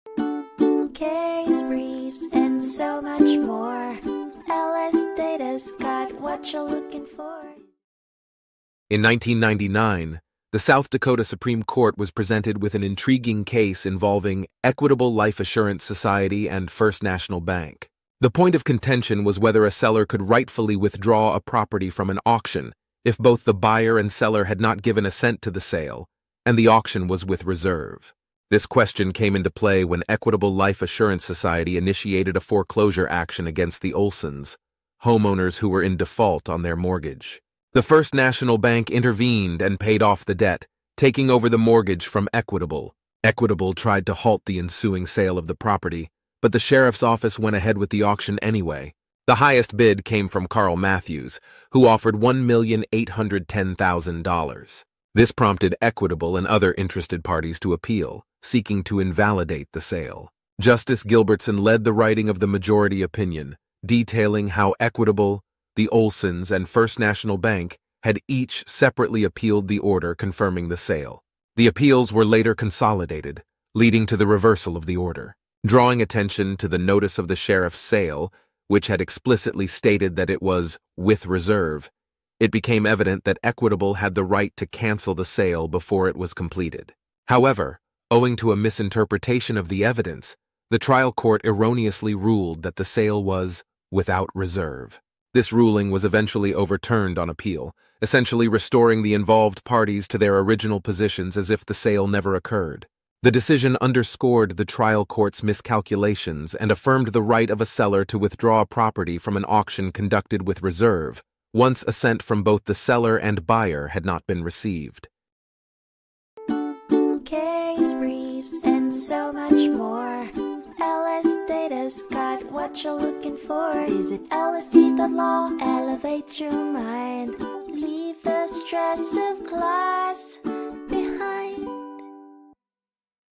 Equitable Life Assurance Society of the United States v. First National Bank (1999) Overview | LSDat
Equitable Life Assurance Society started a foreclosure action against the Olsons, who had defaulted on their mortgage. First National Bank took over the mortgage from Equitable and paid off the debt. Equitable's attorney informed the sheriff's office to call off the sale of the property, but the sale proceeded anyways, and Carl Mathews won with the highest bid of $1,810,000. Equitable and others appealed to set aside the sale.

Equitable Life Assurance Society of the United States v. First National Bank (1999)
South Dakota Supreme Court
602 N.W.2d 291, 1999 SD 144